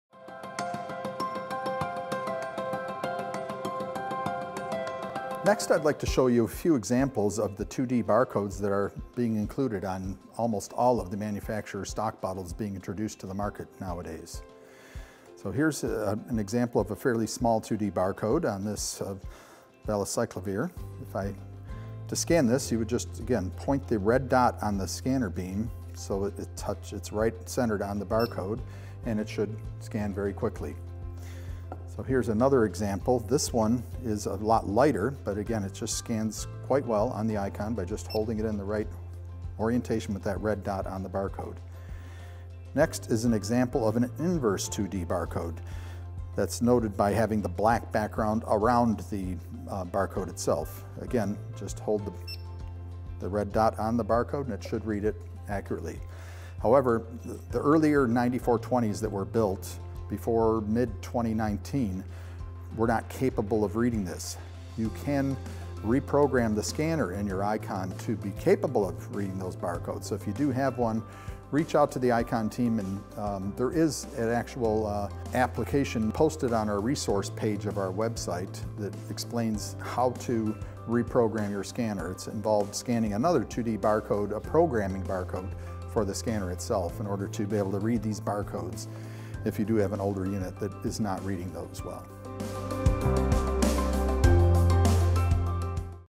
Eyecon 9430 - 2D Barcodes
This video explains what different 2D barcodes look like and how the Eyecon reads and uses them.

If you have any questions or need support, please reach out to our team